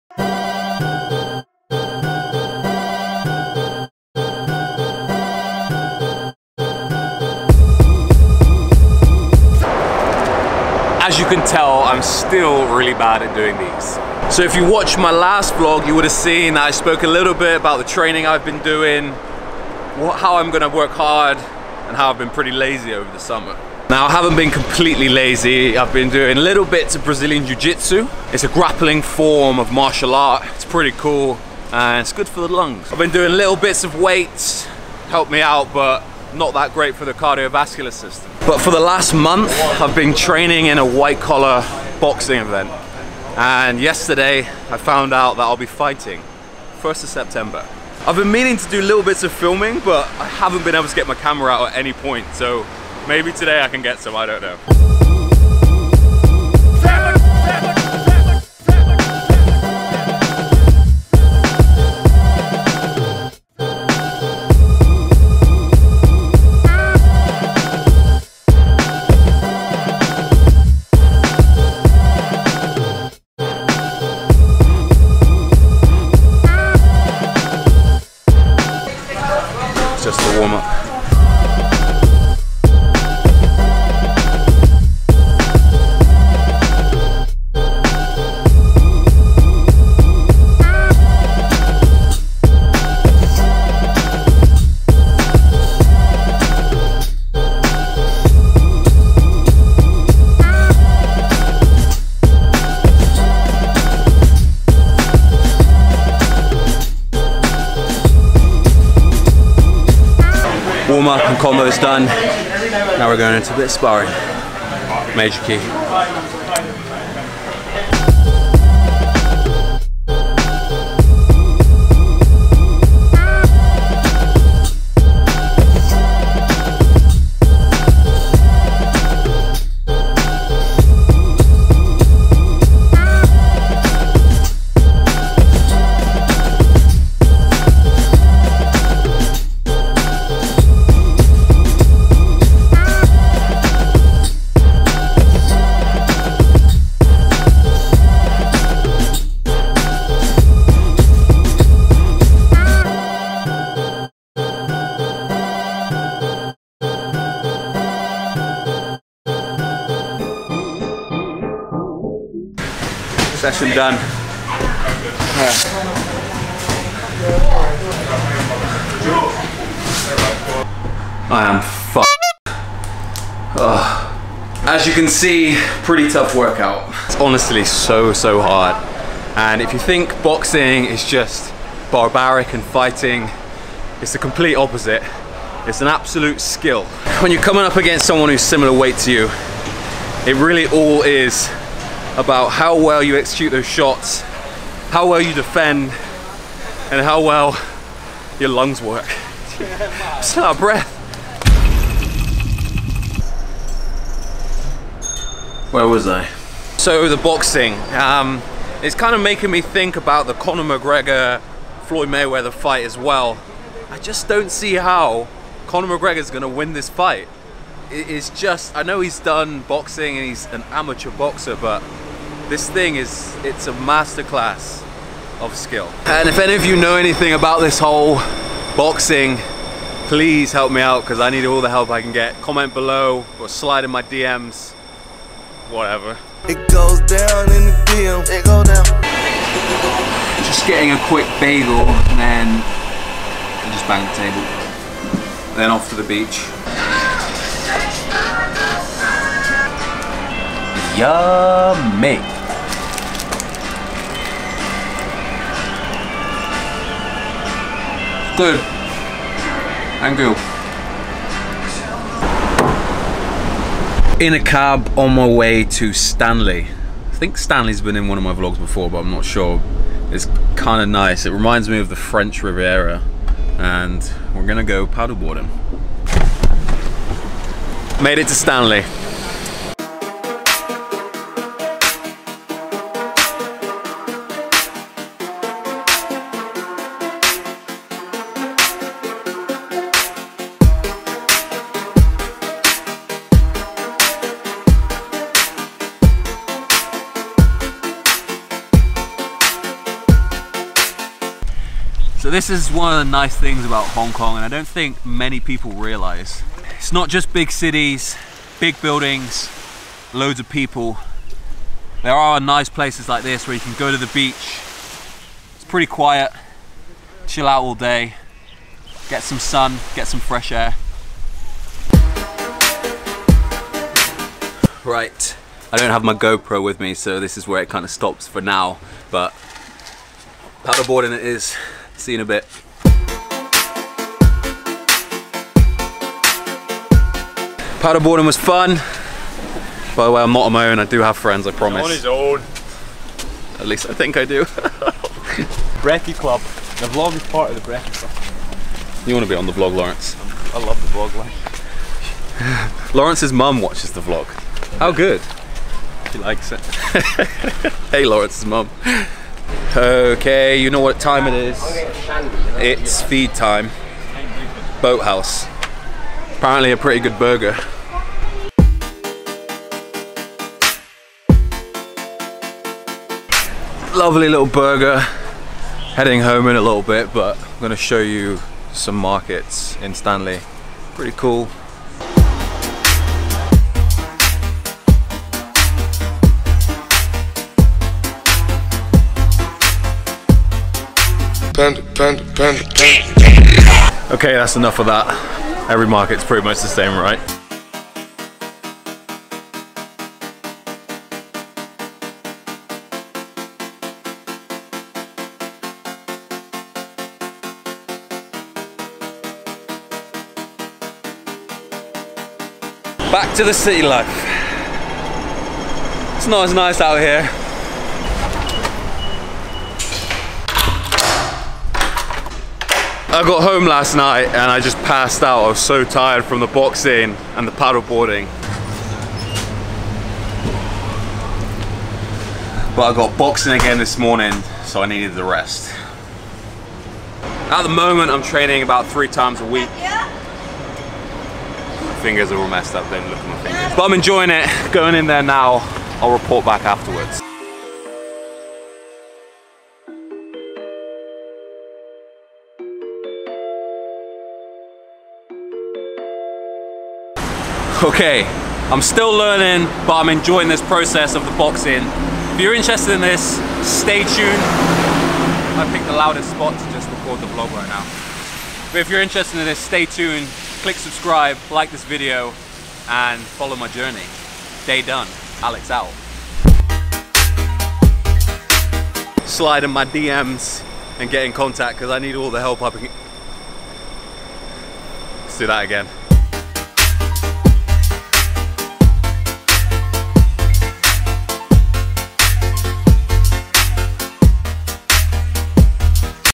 I'M COMPETING IN A WHITE COLLAR BOXING MATCH (watch me train) - 058
**EXPAND FOR ALL INFO, SOCIAL MEDIA & SONGS

FOLLOW ME

SONGS IN THE VLOG——

I appreciate the love!